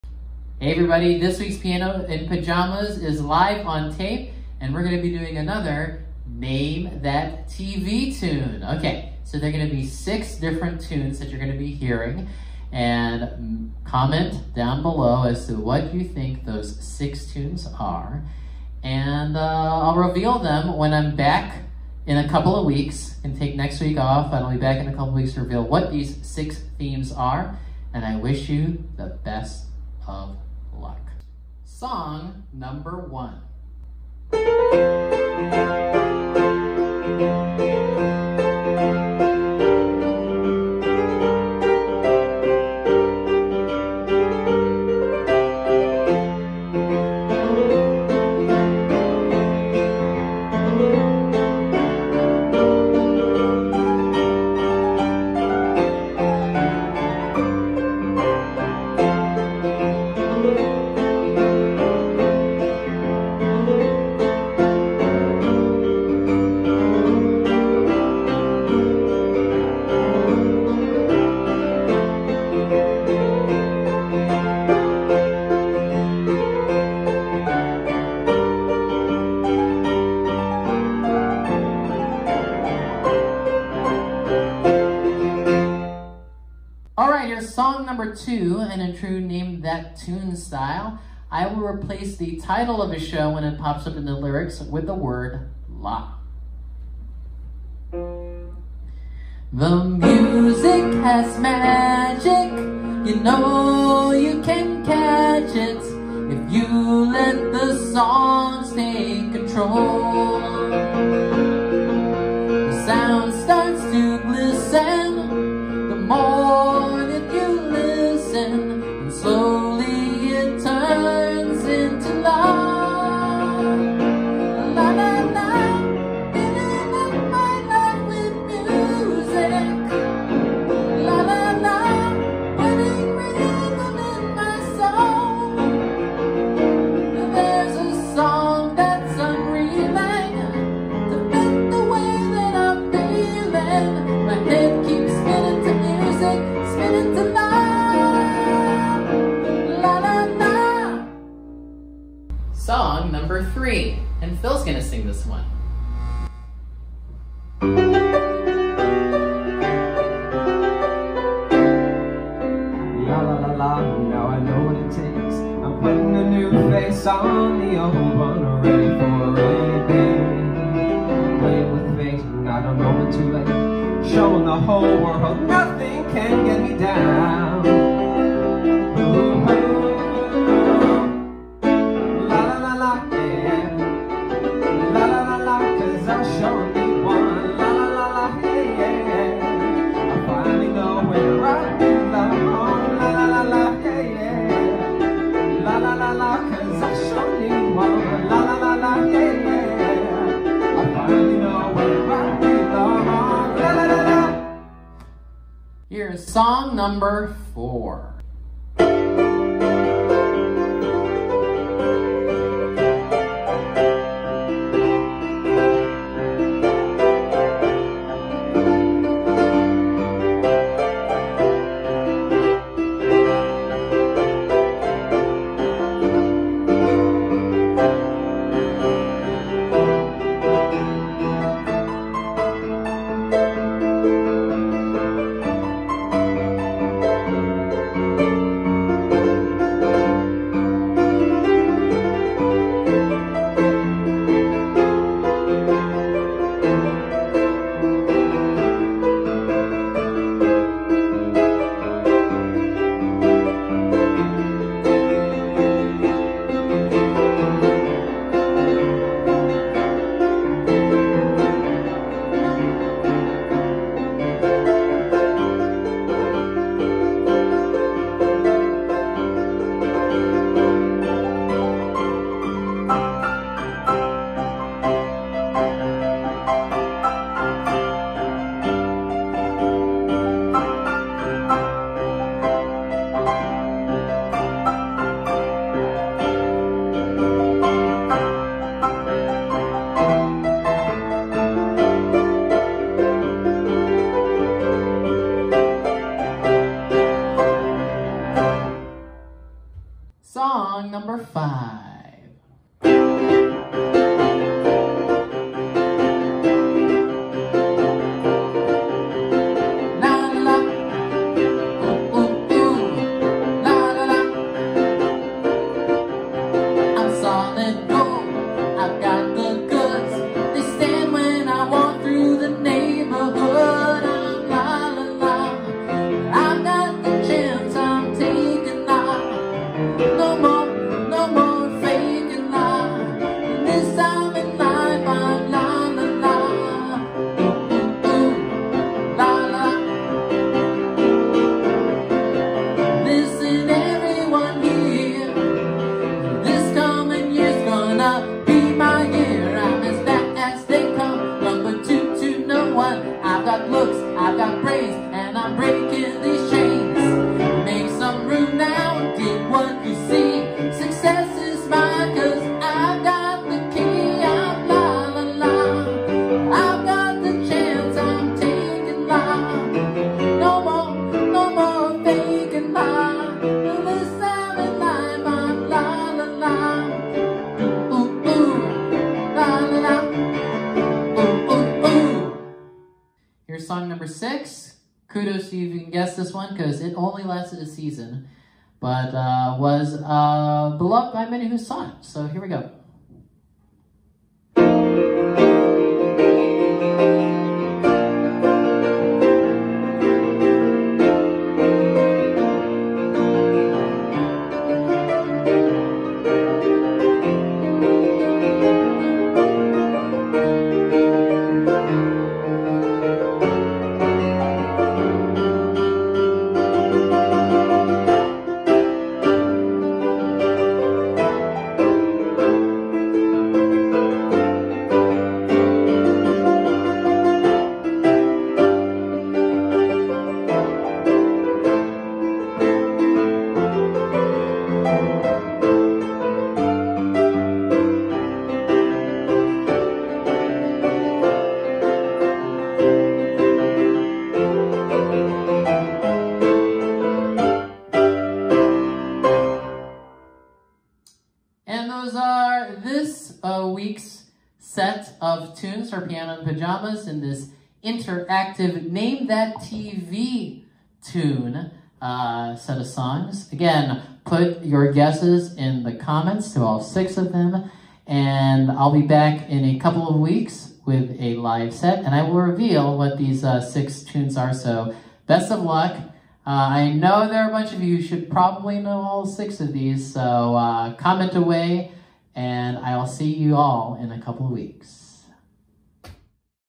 PIANO IN PAJAMAS (8/16/20 - Name That TV Tune II)
This week's PianoInPajamas features a brand new set of mystery TVThemes - let's see if you can identify them! Post your guesses in the comments. I'll reveal the answers when I return on Sunday, August 30th with a new live set

SETLIST
0:00 - Hellos
0:33 - TV Song #1
1:37 - TV Song #2
2:36 - TV Song #3 (w/Phil!)
3:28 - TV Song #4
5:01 - TV Song #5
6:33 - TV Song #6

(13:50 into the video)